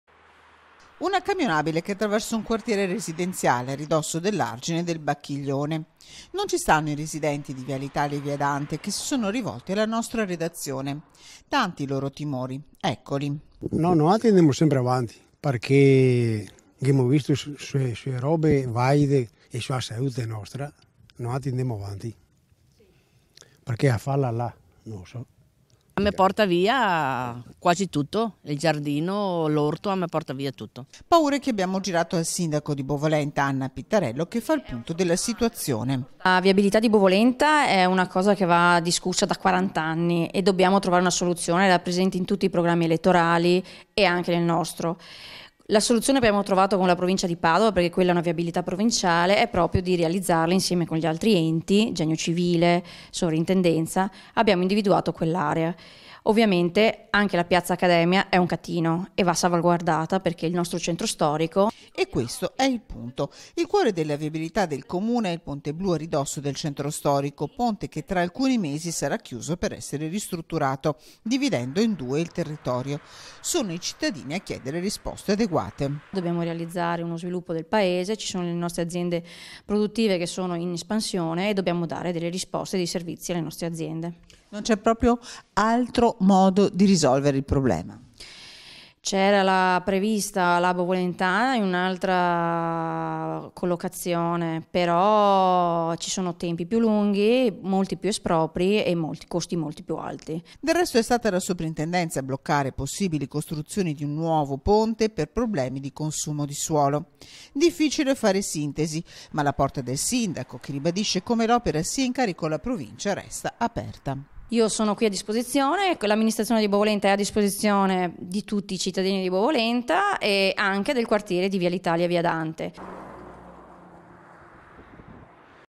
TG (06/02/2019) - CAMIONABILE NEL QUARTIERE. IL SINDACO: «DEVO DARE RISPOSTE AI CITTADINI»
TG (mercoledì 6 febbraio 2019) - Una camionabile che attraversa un quartiere residenziale, a ridosso dell'argine del Bacchiglione. Non ci stanno i residenti di Viale Italia e Via Dante che si sono rivolti alla nostra redazione. Tanti i loro timori, eccoli:
Paure che abbiamo girato al sindaco di Bovolenta, Anna Pittarello, che fa il punto della situazione:E questo è il punto. Il cuore della viabilità del comune è il ponte blu a ridosso del centro storico, ponte che tra alcuni mesi sarà chiuso per essere ristrutturato, dividendo in due il territorio. Sono i cittadini a chiedere risposte adeguate.Del resto, è stata la soprintendenza a bloccare possibili costruzioni di un nuovo ponte per problemi di consumo di suolo. Difficile fare sintesi, ma la porta del sindaco che ribadisce come l'opera sia in carico alla provincia, resta aperta.